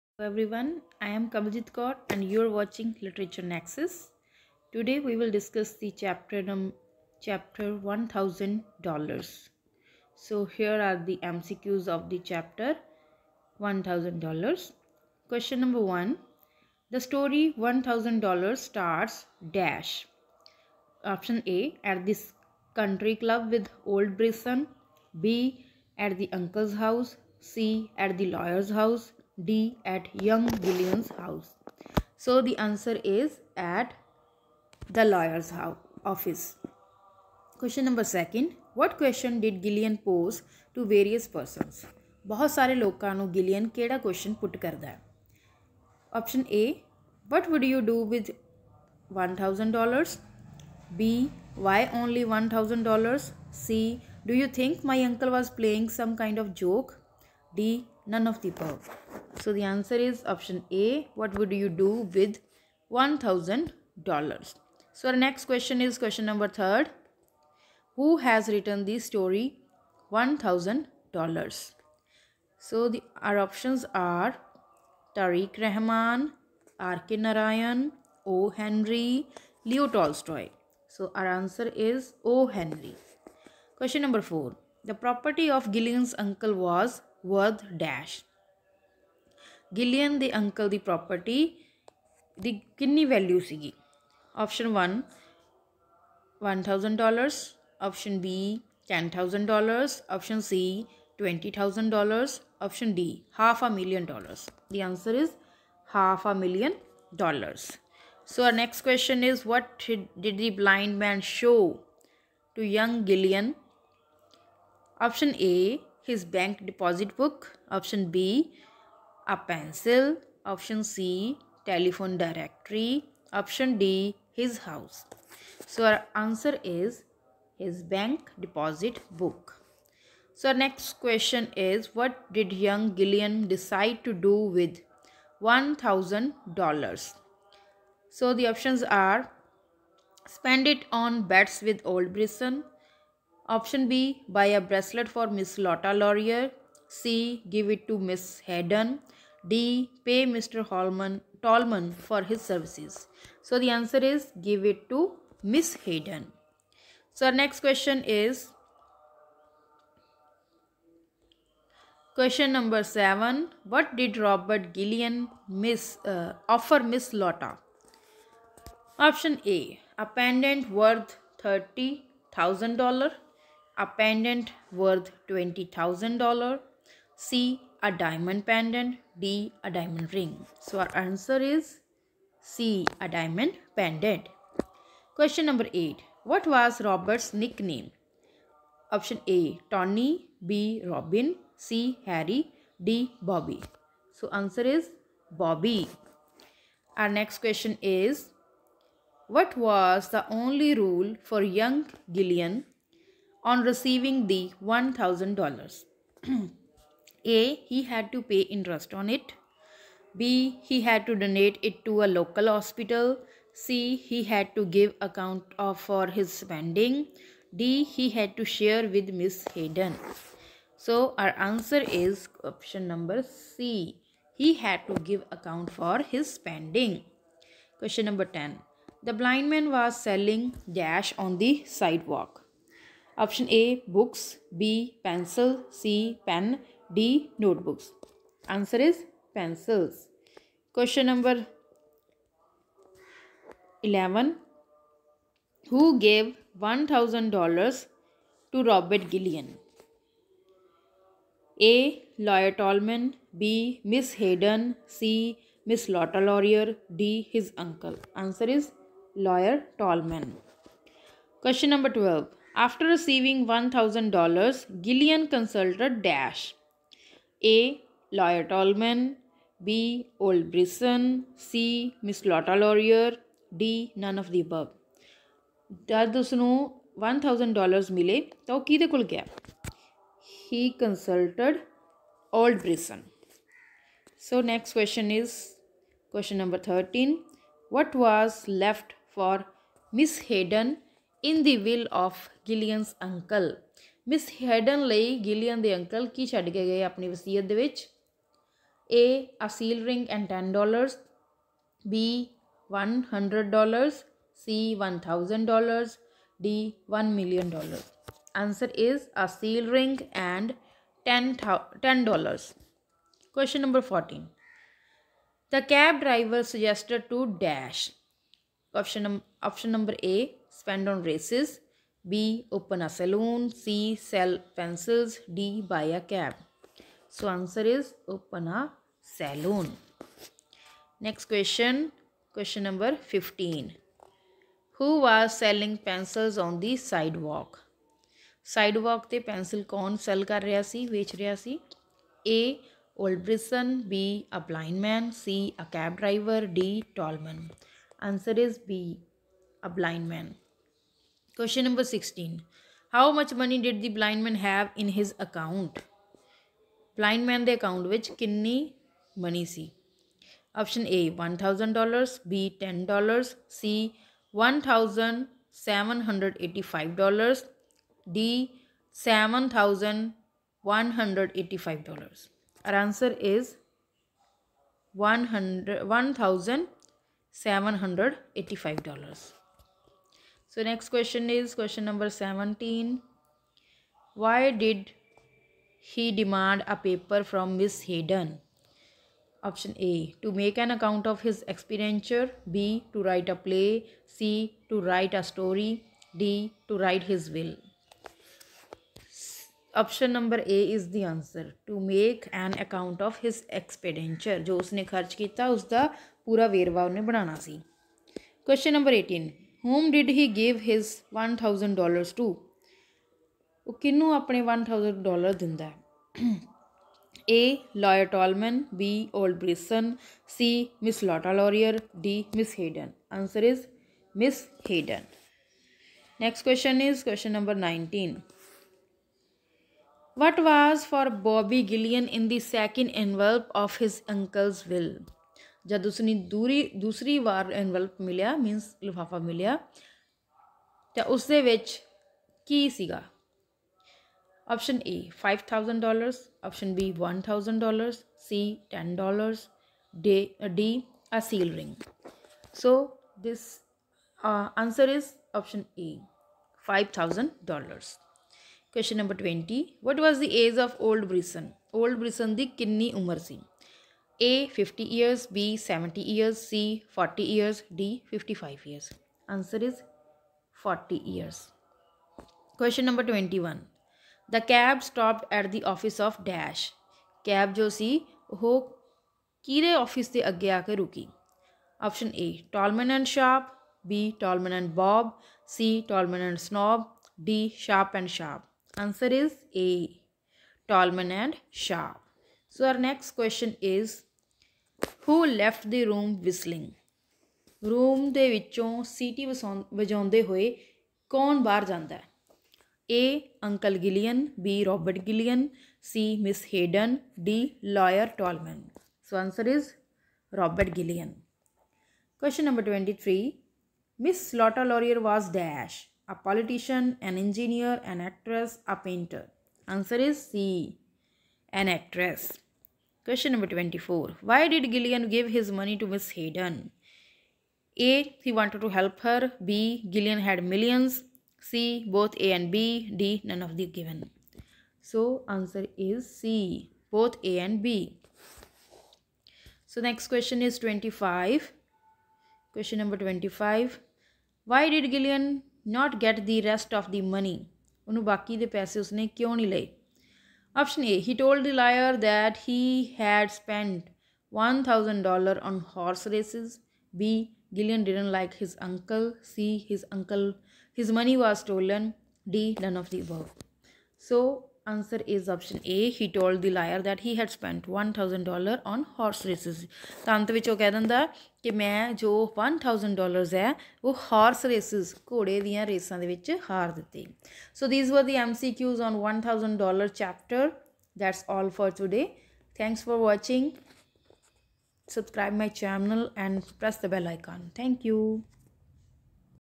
One Thousand Dollars|Chapter-3|class-10th|mcqs|Term-1
Hi ! I'm Kamaljeet Kaur, your English Mistress. I have started the revision of Term 1 syllabus of govt. schools of punjab. Here is a list of some Mcqs on the first chapter.

Keep studing!! Keep watching the Literature Nexus!!